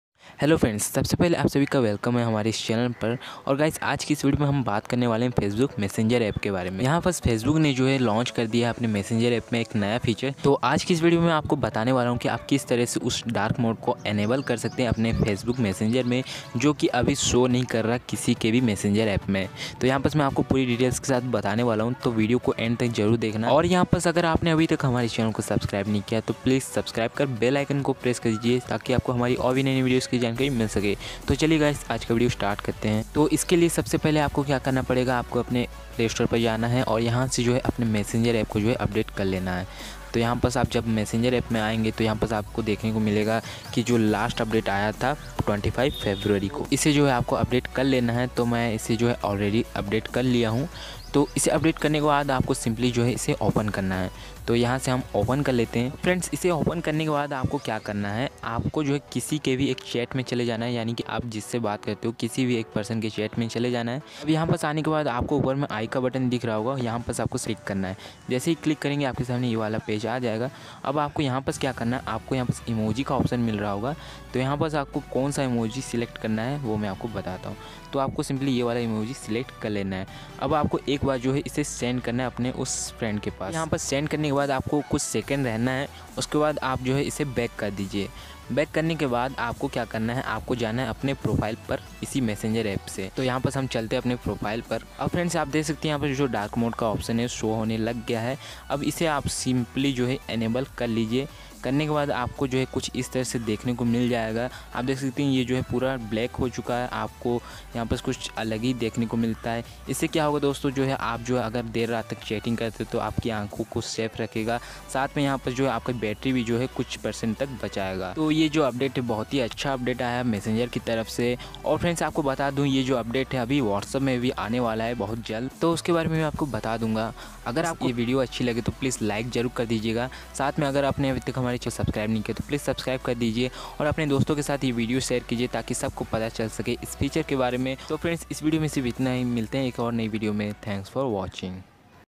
How to enable Dark Mode on Facebook Messenger | Messenger Dark Mode Option Not Showing
Hello Friends. In This Video i will Showing you How to enable Dark Mode on Messenger. I Hope You Like This Video 💕

My Setup
Mobile ( Realme1 4/64)

Tech Media Channel Ko Support Kijiye aur Niche diye Huye Link Se Purchase Kijiye